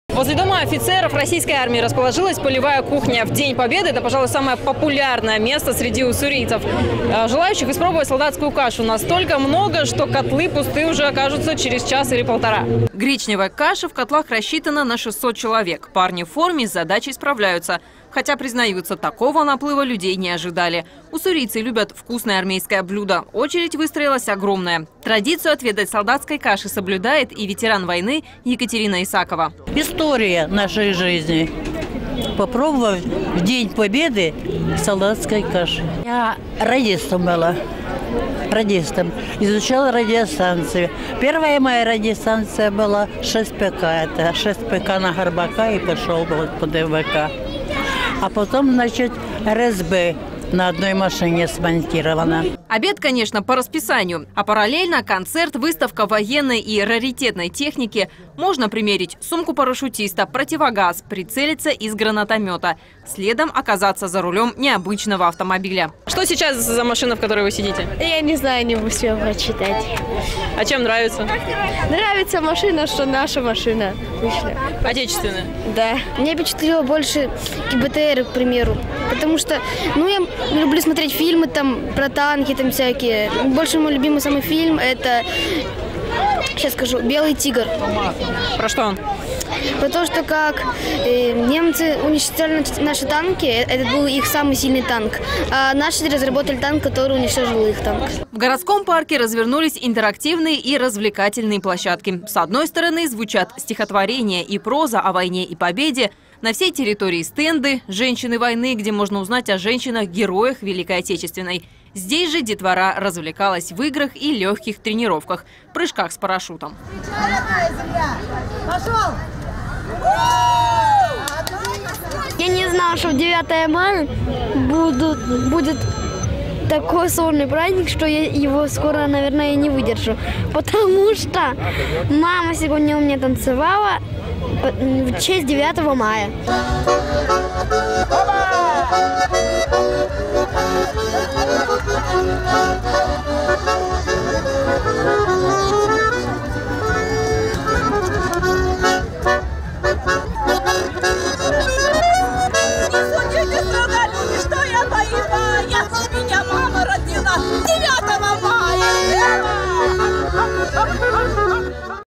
Солдатскую кашу в День Победы уссурийцы съели за час
Возле Дома офицеров российской армии 9 мая расположилась полевая кухня. В День Победы - это, пожалуй, самое популярное место среди уссурийцев. Желающих попробовать солдатскую кашу настолько много, что котлы оказались пустыми через час.
Наш канал в Telegram по-прежнему